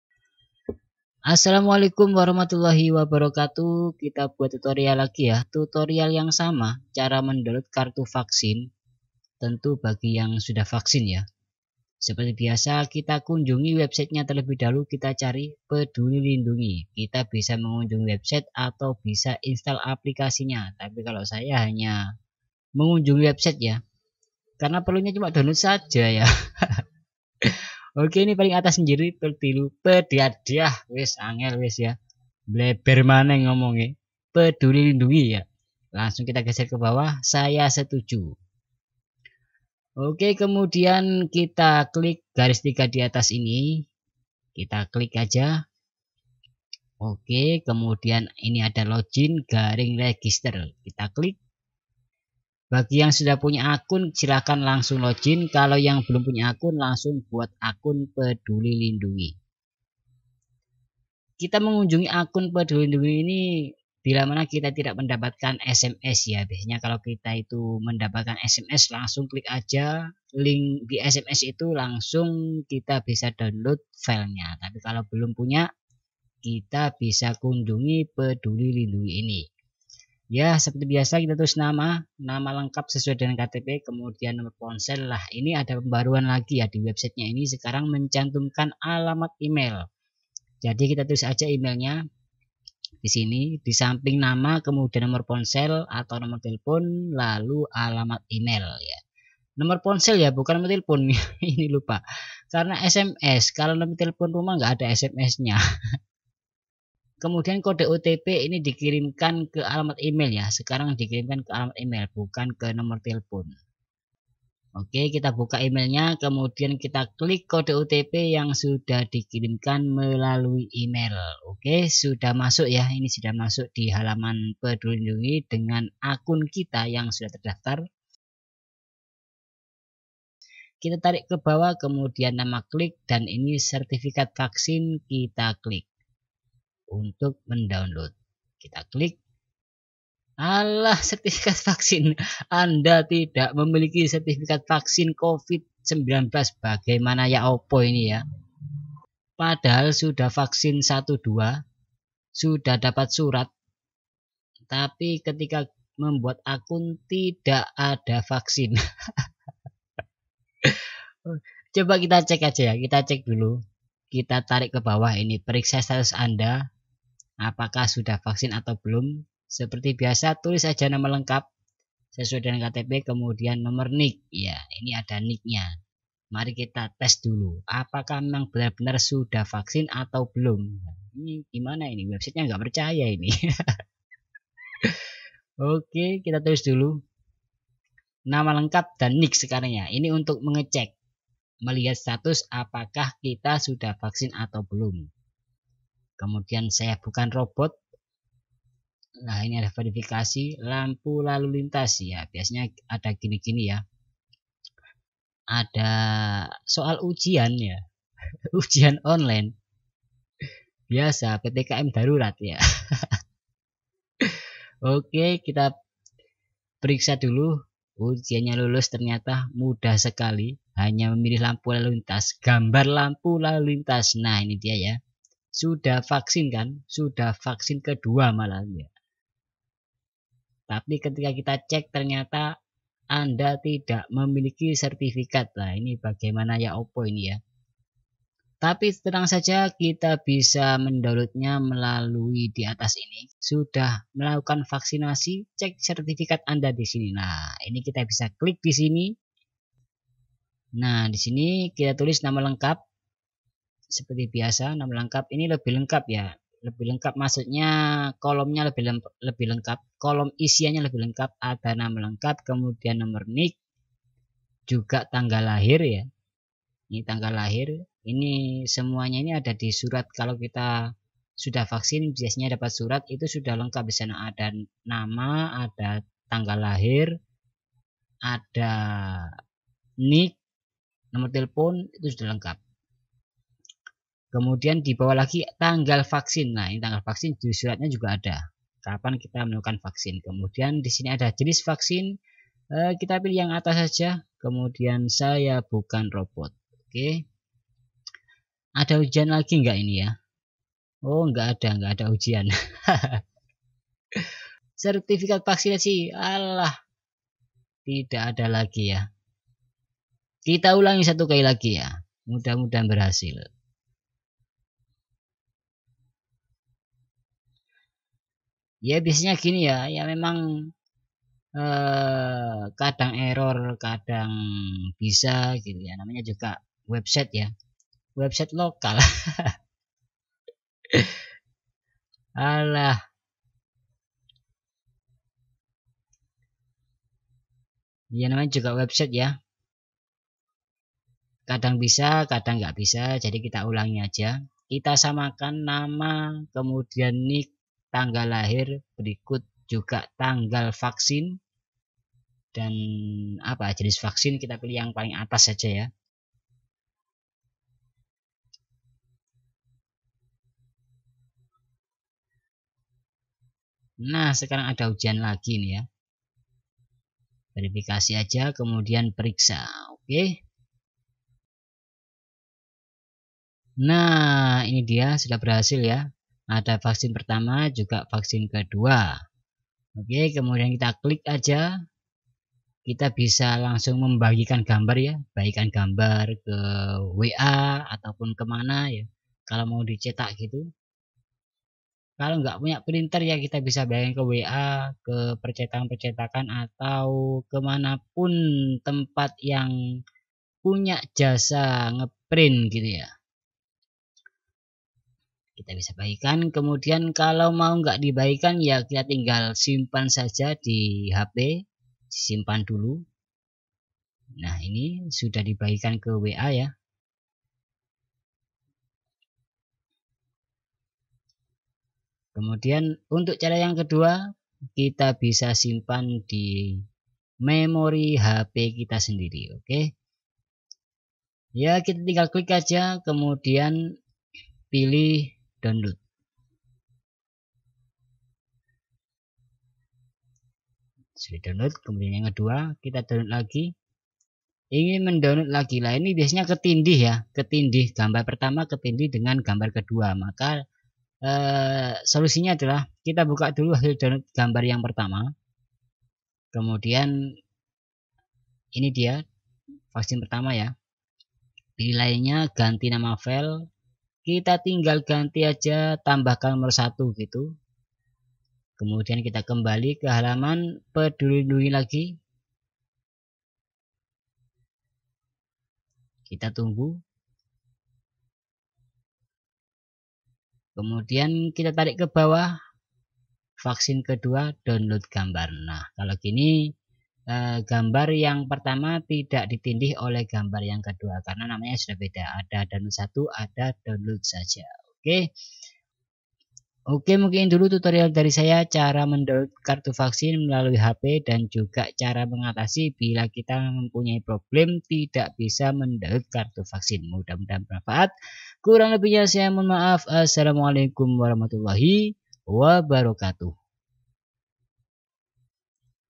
solusi tidak bisa download kartu vaksin
solusi tidak bisa download kartu vaksin. Kadang di web tertulis tidak ada sertifikat vaksin, padahal sudah vaksin yang kedua dan dapat surat.
Berikut cara download kartu vaksin bila menemui kendala seperti di atas. semoga bermanfaat